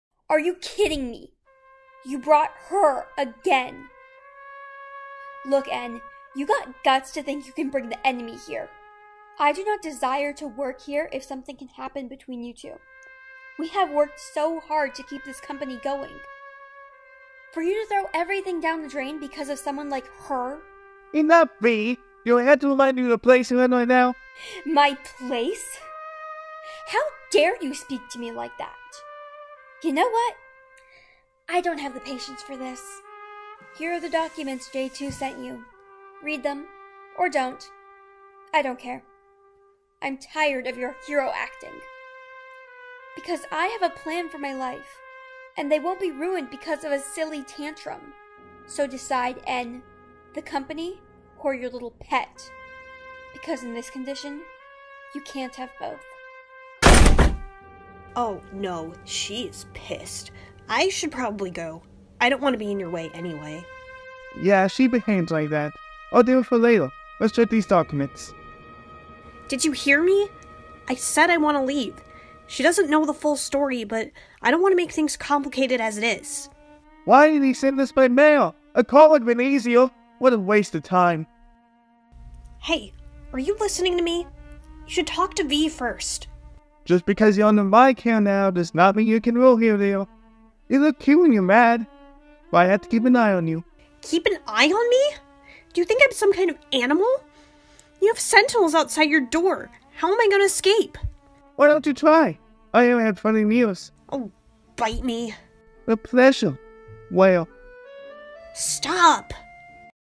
N Starts A Mafia Business With Uzi | Murder Drones Comic Dub
In this Murder Drones Comic Dub, After trying to help Uzi uncover her new Powers, N Accidentally Reveals His True Feelings About Uzi. How will Uzi respond to N's Accidental Reveal? Find out in this Romantical Comic Dub

🎙️People Involved 🎙️

Will Starshine as N

🎨 Comic Featured 🎨:

N Accidentally Reveals His True Feelings About Uzi is by ItsVanilles on Instagram: